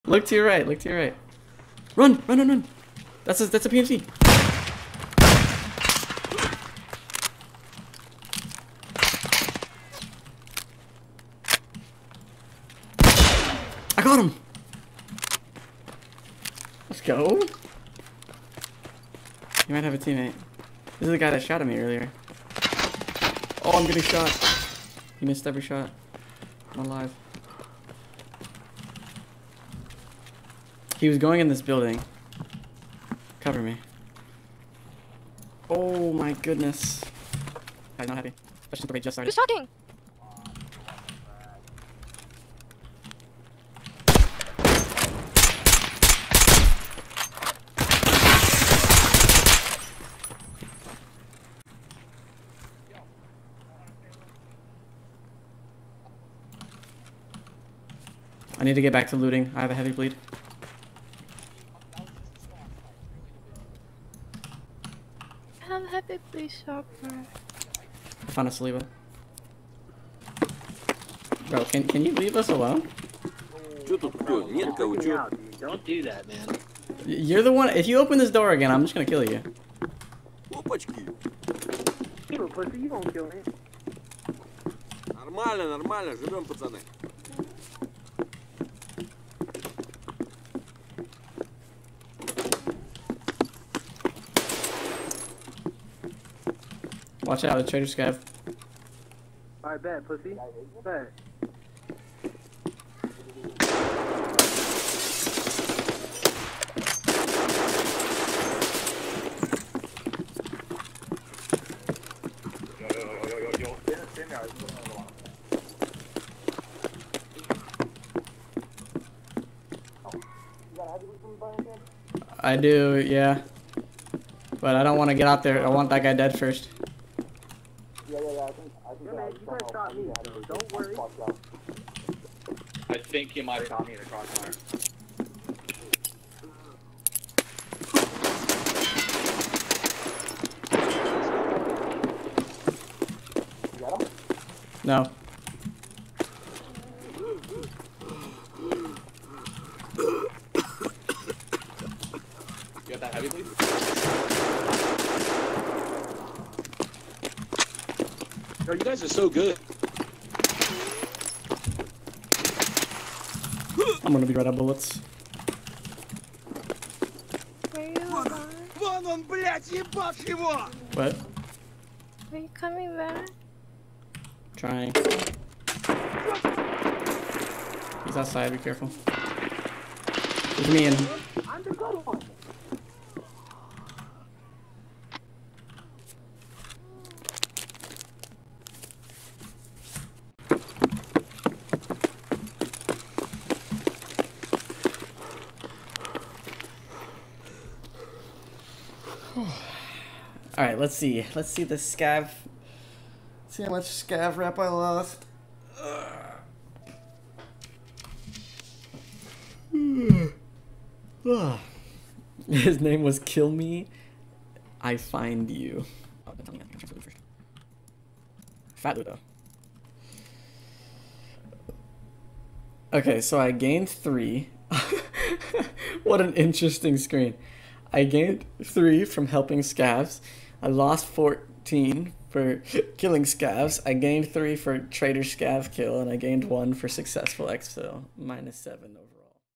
Scav on Scav Violins
Scav Rep went all over the place with this one. This guy saw that I killed a pmc, had good loot, and would NOT leave me alone. Don't be like this guy. Either shoot me straight up or just fk off and mind your business.